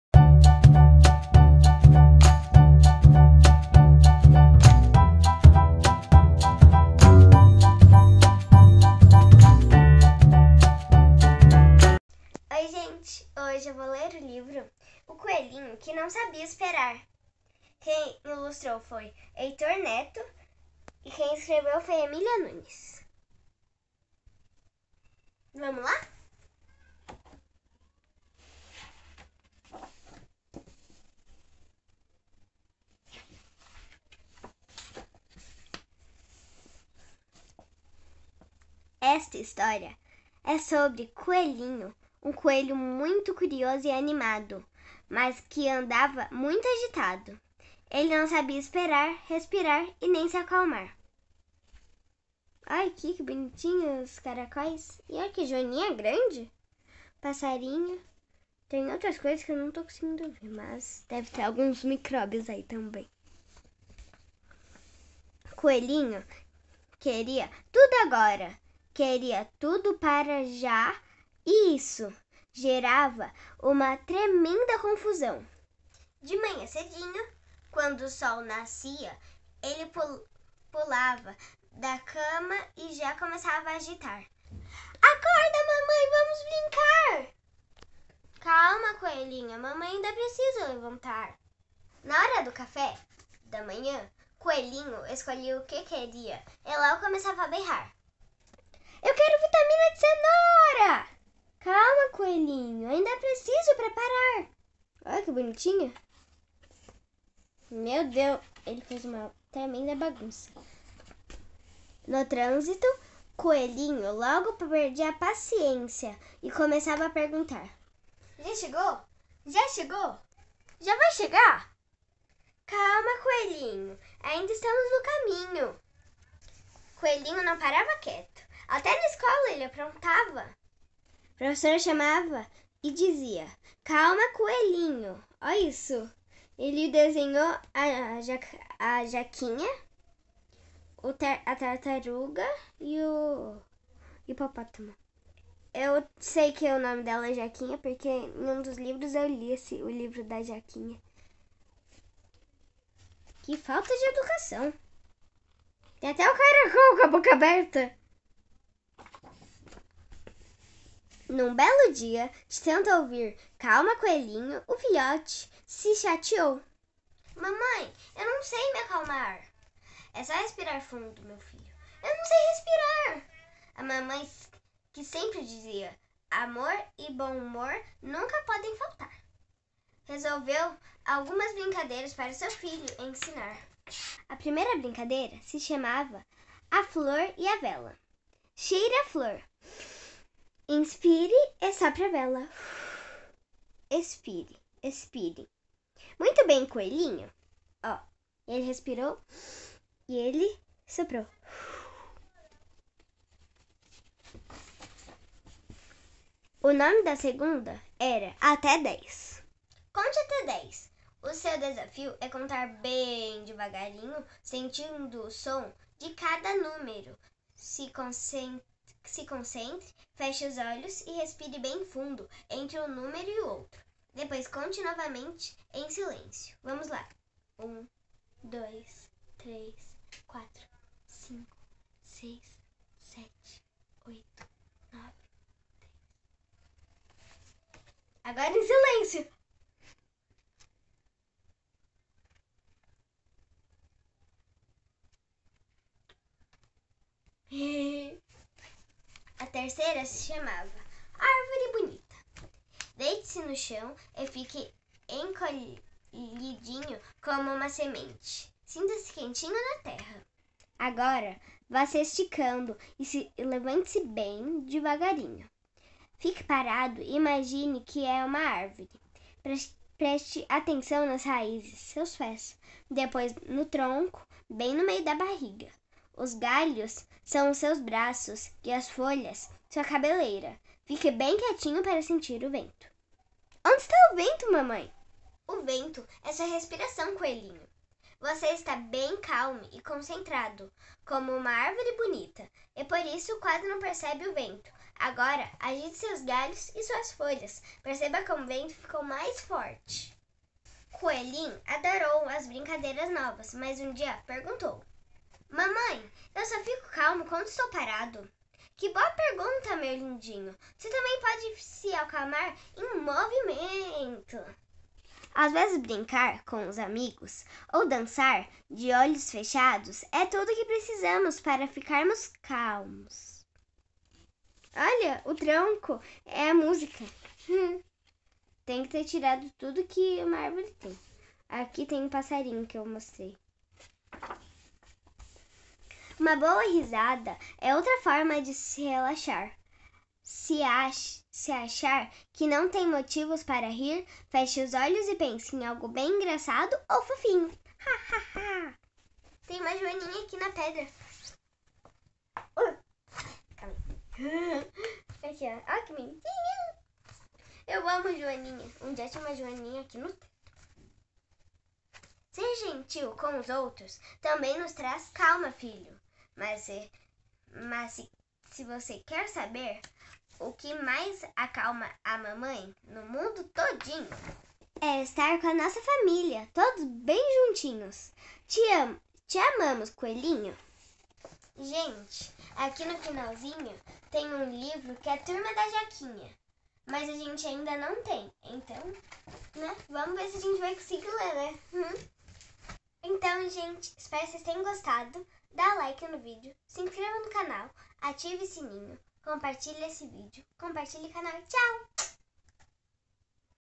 O COELHINHO QUE NÃO SABIA ESPERAR | EMÍLIA NUÑEZ - HISTORIAS QUE A ISA CONTA
O livro de hoje fala de um coelhinho muito apressado!!! Quer saber como ele conseguiu se acalmar? Assista esse video!!!

Texto: Emília Nuñez
Ilustrado: Heitor Neto
Editora: Tibi Livros

*Conteúdo livre para todas as idades.